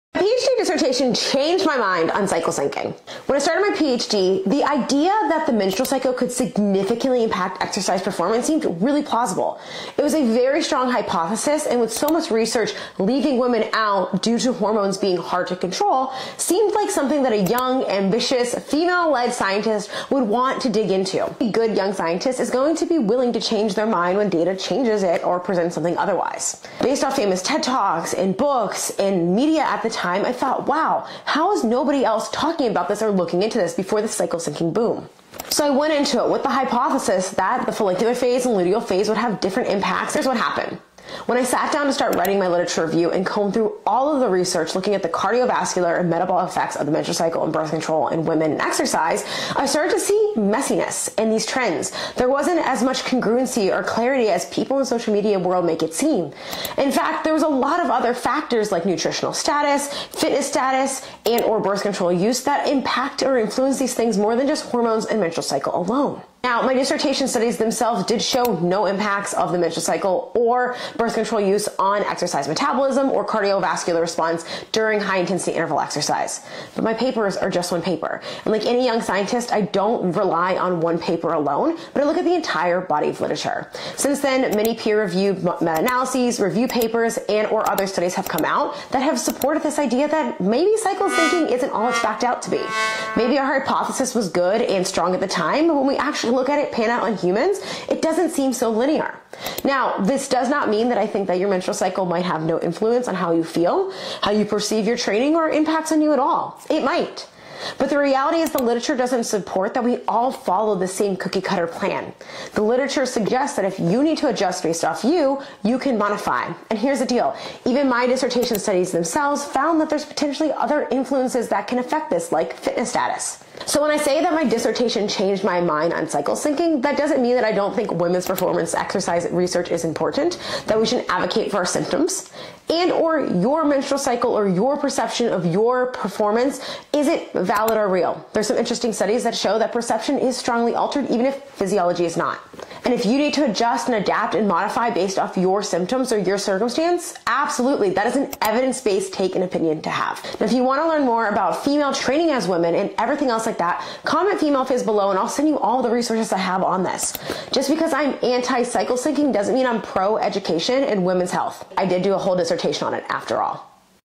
Story time: how my PhD in exercise, metabolism & women changed my mind on “cycle based training”
My most vital reels this year (one at 1.1M+) have been about this — with plenty of people angry at me over this stance or not being “more clear” about what my original views were vs. are now. So let’s sit down for some story time and clear this up!

have you ever changed your mind when learning new or more information? 👇🏻